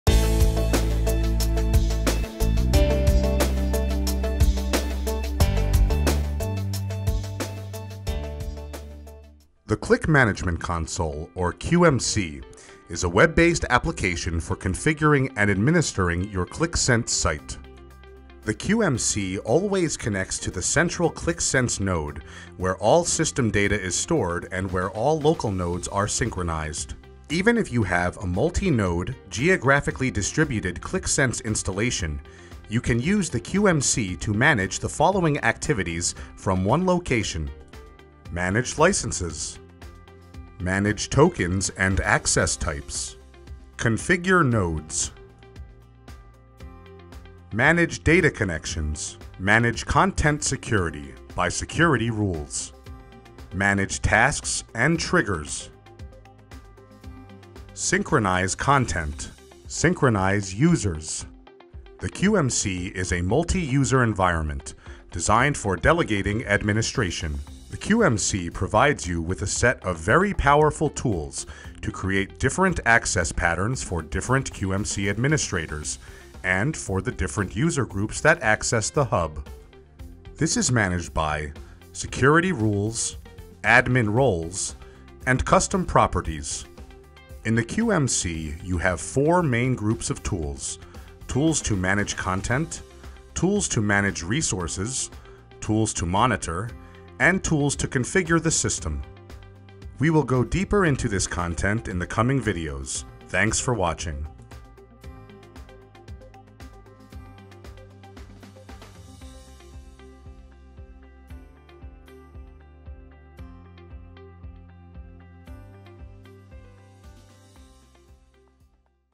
Understanding QMC - Qlik Sense
This video is a quick introduction to the Qlik Management Console (QMC). The QMC is a web-based application for configuring and administering your Qlik Sense site. The QMC provides you with a set of very powerful tools to create customized access patterns for different QMC administrators and for the various user groups that access Qlik Sense hub.